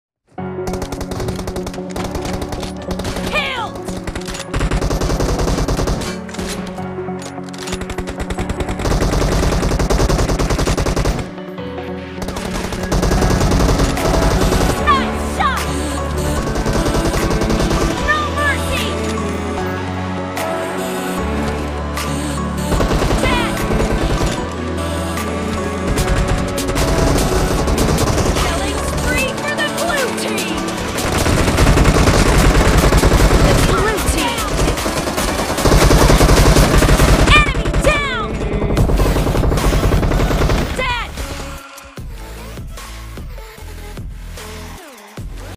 TDM M24 SNIPING PART-2 | M24 King
A 15 year old Indian🇮🇳Playing for skill improvement not for kd and tier.

Controls - 4 finger claw + Full gyroscope( Always on)

Server - Asia

Style - soft

Season - 15

Recorder - Inbuilt

Editiing software - Kinemaster